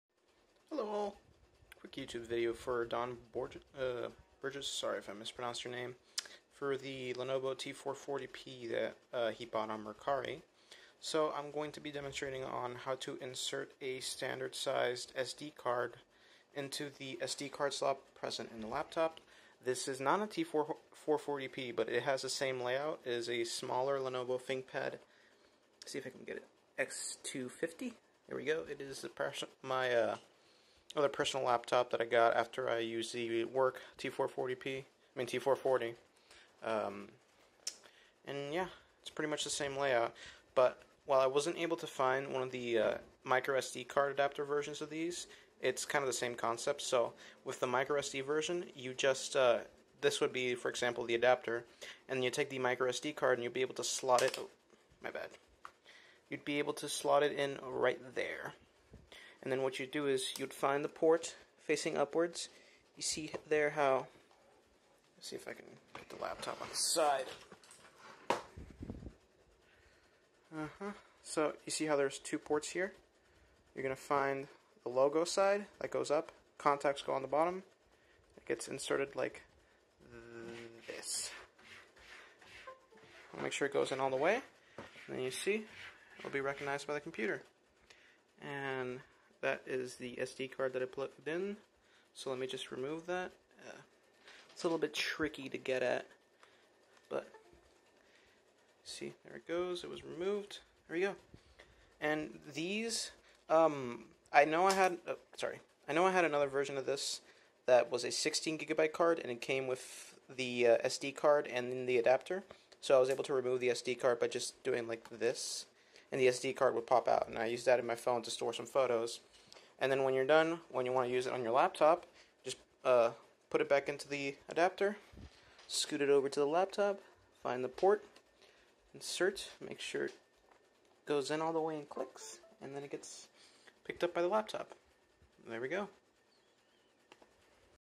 Lenovo Thinkpad, SD card tutorial.
This is a small explanation video where I talk a little bit about how to use a standard sized SD card in the lenovo Thinkpad T440/x250 laptop (both use same arrangements), The micro SD card would require the adapter that comes in the package alongside the microSD on most models, and it is the same size as the SD card used in the video, the only difference is that you insert your smaller MicroSD card into the larger sized SD adapter.